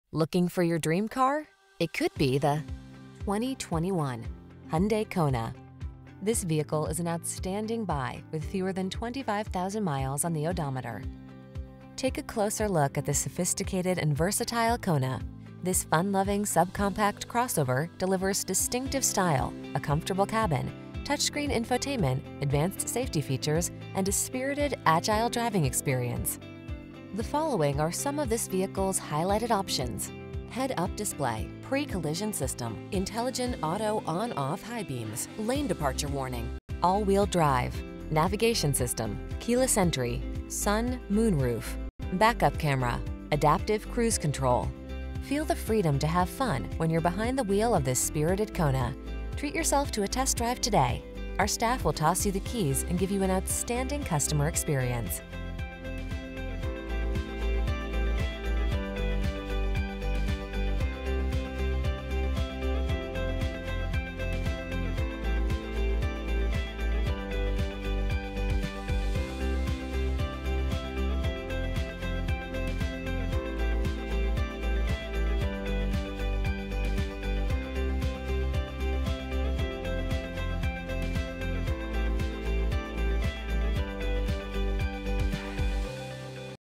2021 Hyundai Kona Ultimate CO Denver, Arvada, Lakewood, Bloomfield, Thorton
Pre-Owned 2021 Hyundai Kona Ultimate available at Phil Long Ford of Denver located in 7887 West Tufts Avenue, Denver, CO, 80123. Servicing the Denver, Arvada, Lakewood, Bloomfield, Thorton area.
Please mention the stock E00675A when you contact the dealership for this vehicle.